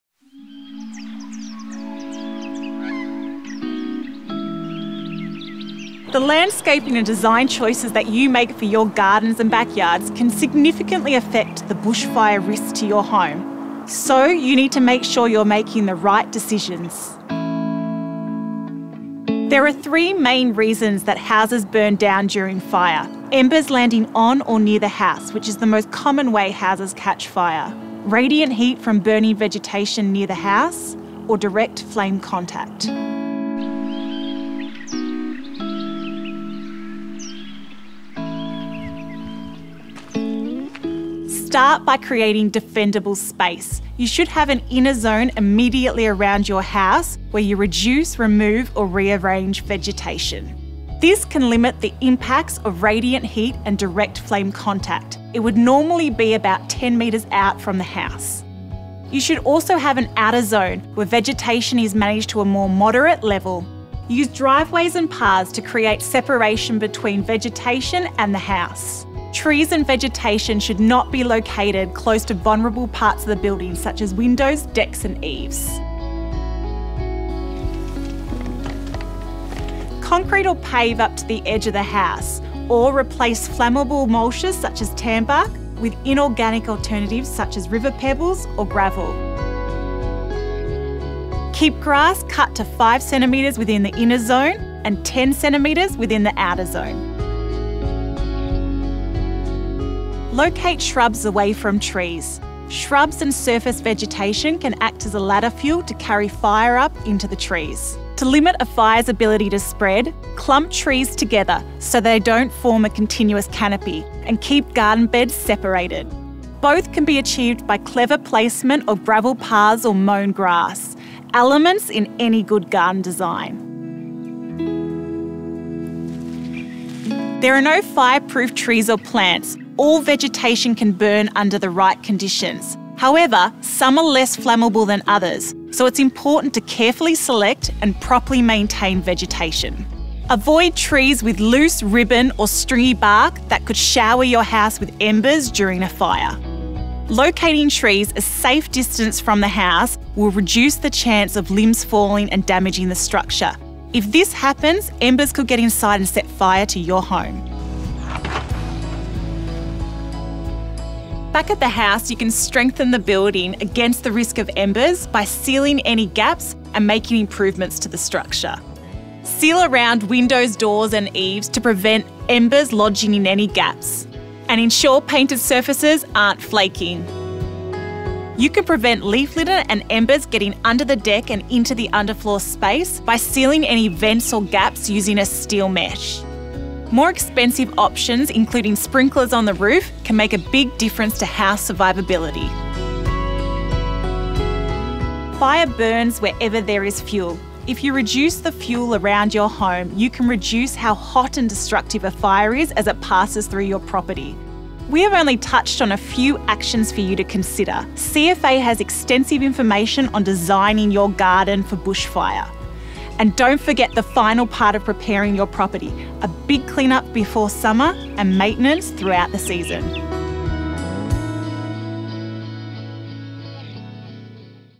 Designing your garden for bushfire
Learn about landscaping and design choices you can make to reduce the bushfire risk to your home.

This video was developed for the Property Preparation module which is delivered in CFA's Fire Safety Programs